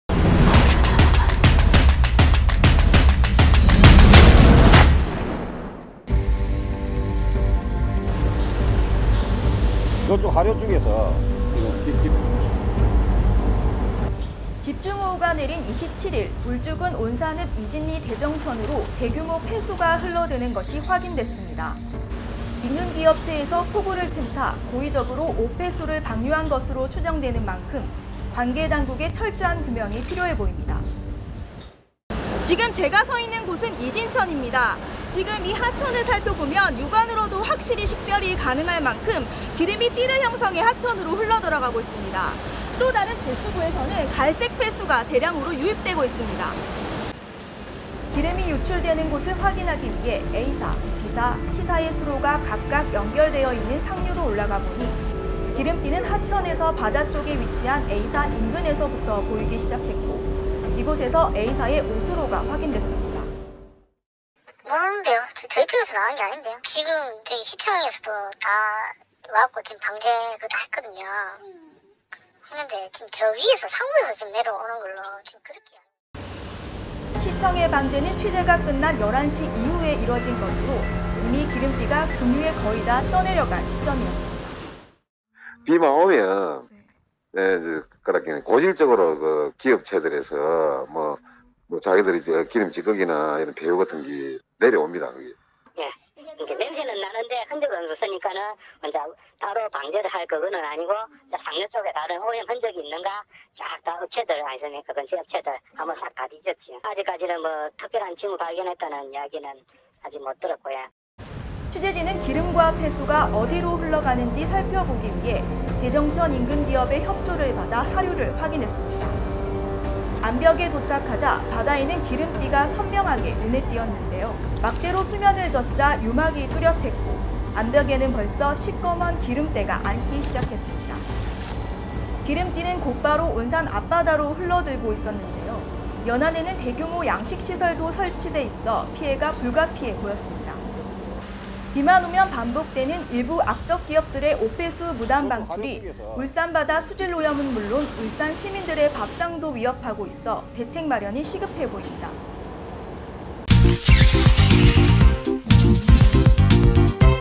[UTV 현장출동] 아직도 비만 내리면 바다로 오·폐수 '콸콸'?!
비만 오면 반복되는 일부 악덕 기업들의 오·폐수 무단 방출이 울산 바다 수질 오염은 물론 울산 시민들의 밥상도 위협하고 있어 대책 마련이 시급하다. (취재 : 신섬미 기자, 촬영·편집 : 고태헌 기자, 제작 : 울산매일 UTV)